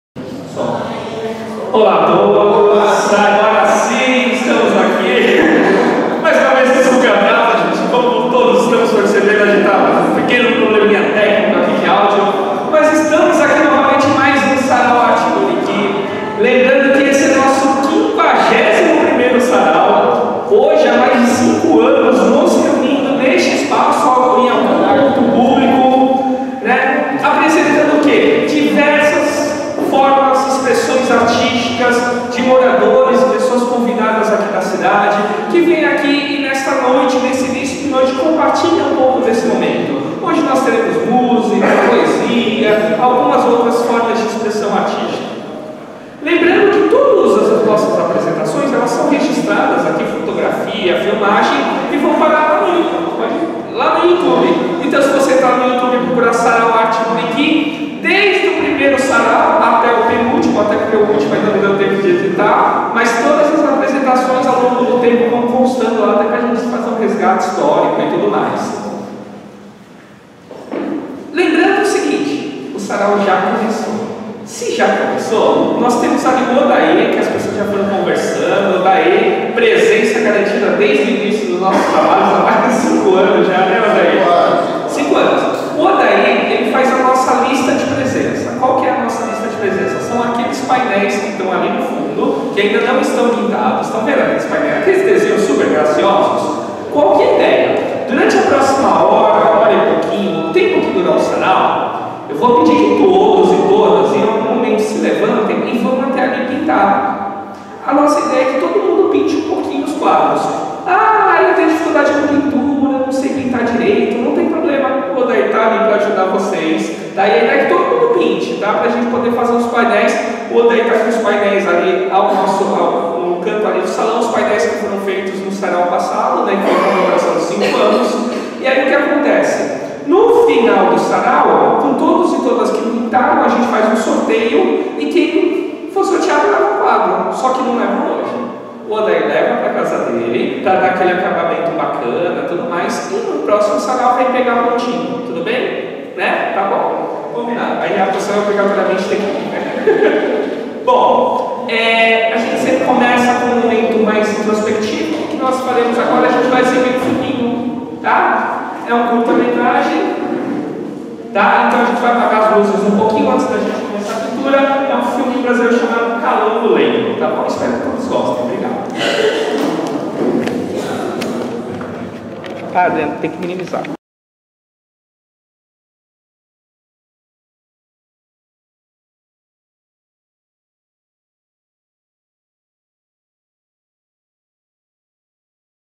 51º Sarau Arte Buriki - Rogério Wong - Boas Vindas e Apresentação (1ª parte)
51º Sarau Arte Buriki, 1ª parte, realizado em Setembro de 2017, pelos munícipes de Bertioga e convidados, nasceu como uma proposta do Projeto Coração Selvagem (hoje Banda Tupiniquins Roll) e que conta com a parceria da Casa de Cultura de Bertioga.